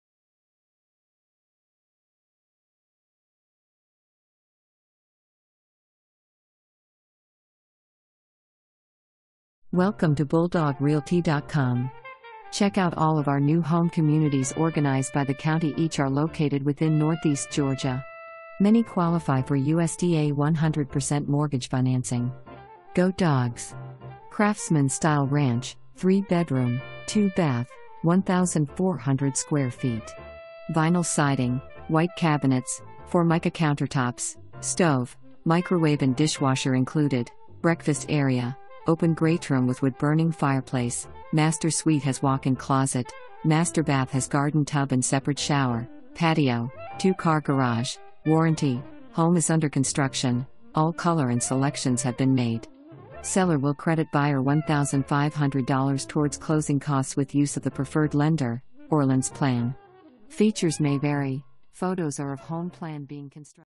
142 Rolling Woods Lane
142 Rolling Woods Lane
Hull, GA 30646

Craftsman style ranch, 3 bedroom, 2 bath, 1400 sq ft. Vinyl siding, white cabinets, formica countertops, stove, microwave and dishwasher included, breakfast area, open greatroom with wood burning fireplace, master suite has walk in closet, master bath has garden tub and separate shower, patio, 2 car garage, warranty, home is under construction, all color and selections have been made! Seller will credit buyer $1500 towards closing costs with use of the preferred lender, Orleans Plan! Features may vary, photos are of home plan being constructed, not of actual subject property! Home is in beginning stages of construction 60 to 75 days to complete!

2 full bath

Charles Williams
BulldawgRealty.com